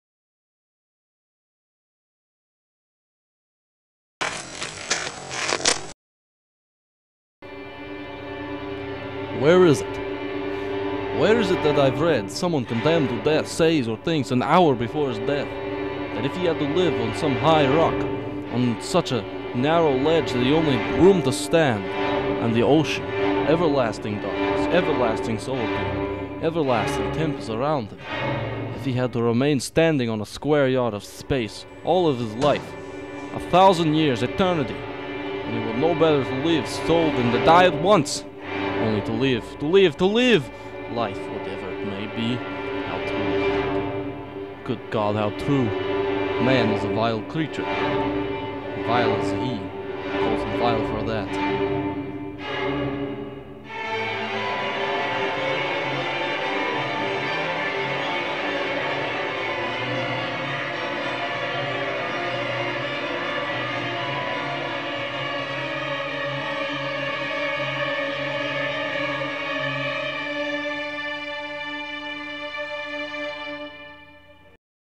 Crime&Punishment Trailer
A trailer for Dostoevsky's "Crime and Punishment." Another A.P. English Project, Enjoy!